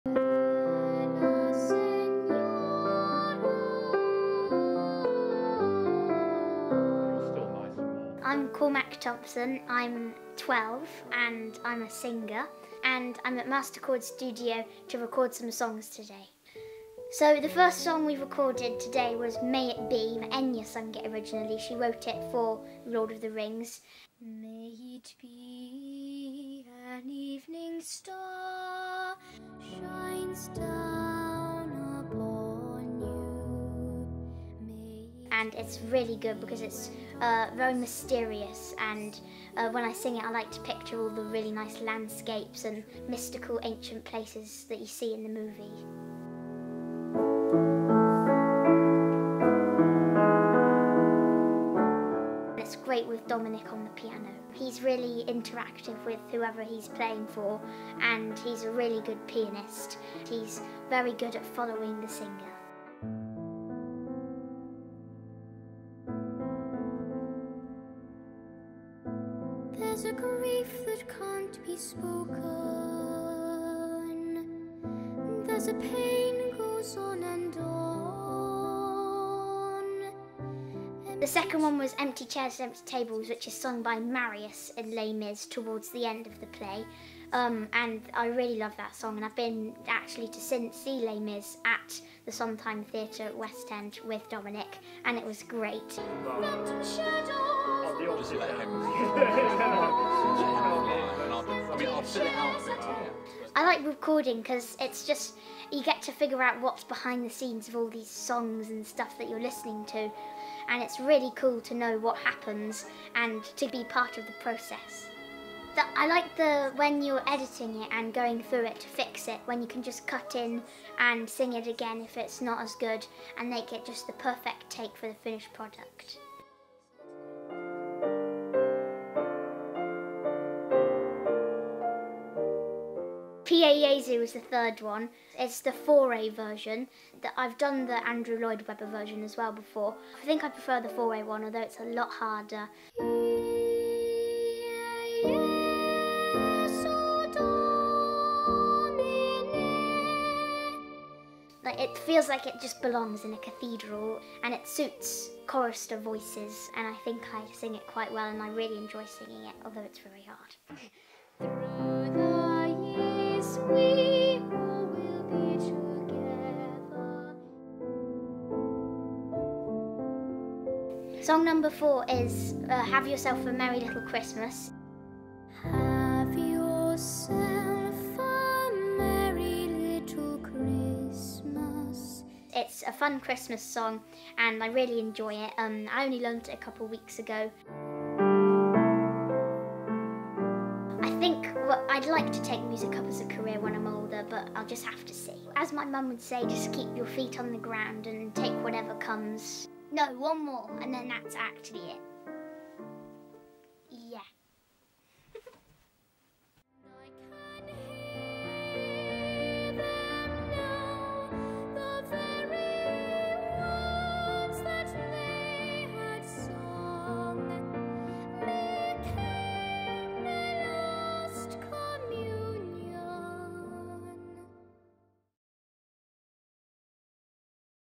In my own words. Behind the scenes chats from the studio.
Only a little bit of singing on this one, I'm talking for a change  😊

Piano: Dominic Ferris
Engineered and Mixed by: Simon Hanhart
Recorded at Masterchord Studio, London
Filmed and edited by: Dino Kazamia
Video assistant Jackson Buckler
Funded: Independently, as not signed to a record label